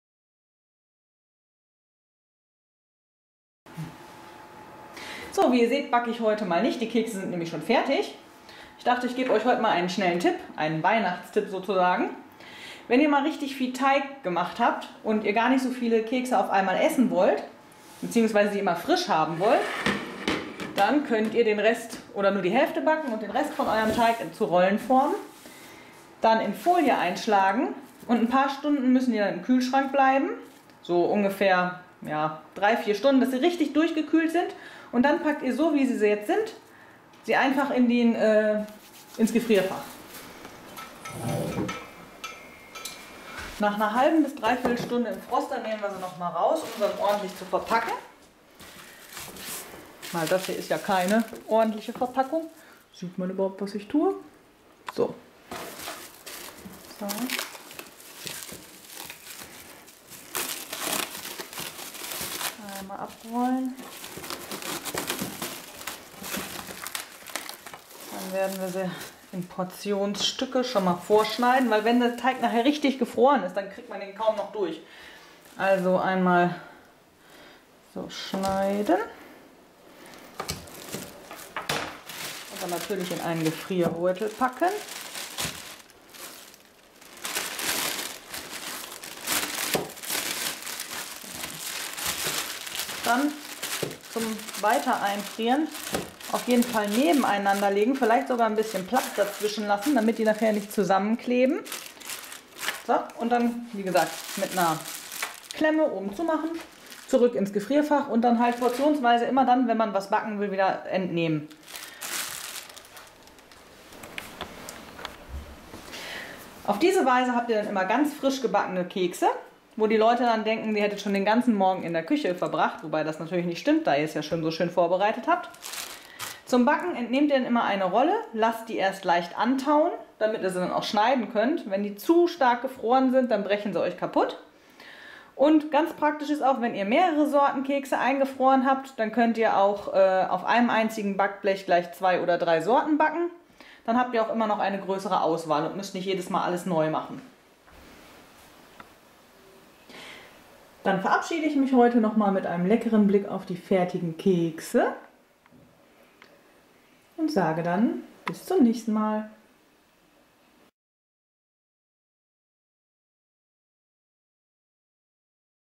Plätzchenteig einfrieren / Weihnachtstipp
Wenn ihr verschiedene Plätzchenteige einfriert habt ihr immer mehrere Sorten vorrätig um sie frisch aufzubacken.
Das macht dann auch viel weniger Arbeit und ihr habt eine große Auswahl.